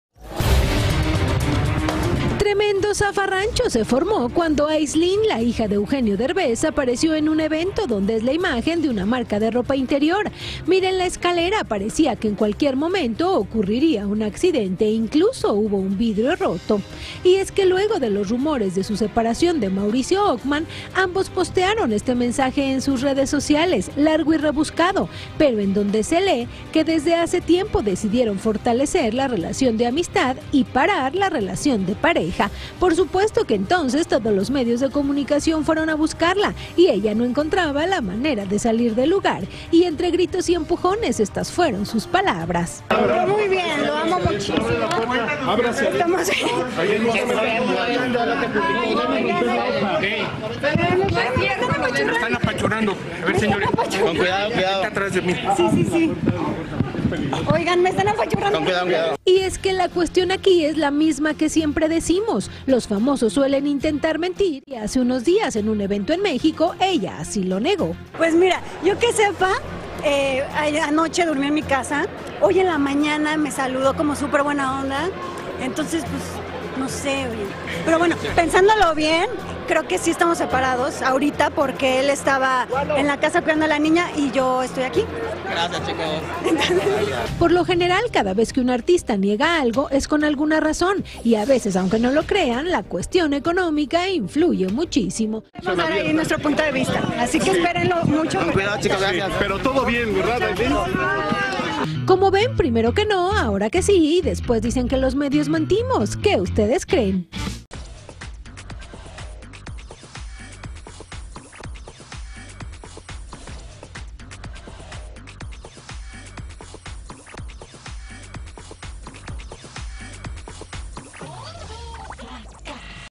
Ya no están juntos, pero hace unos días Aislinn Derbez negó su separación de Mauricio Ochmann | GYF
La hija de Eugenio Derbez apareció en su primer evento público tras anunciar que ella y al actor no siguen juntos como pareja. Su presencia provocó un caos entre los medios de comunicación, y ante los gritos y empujones, prefirió evadir cualquier pregunta sobre su ruptura. Sin embargo, hace pocos días fue la propia Aislinn quien negó los rumores de su separación y aquí recordamos sus palabras.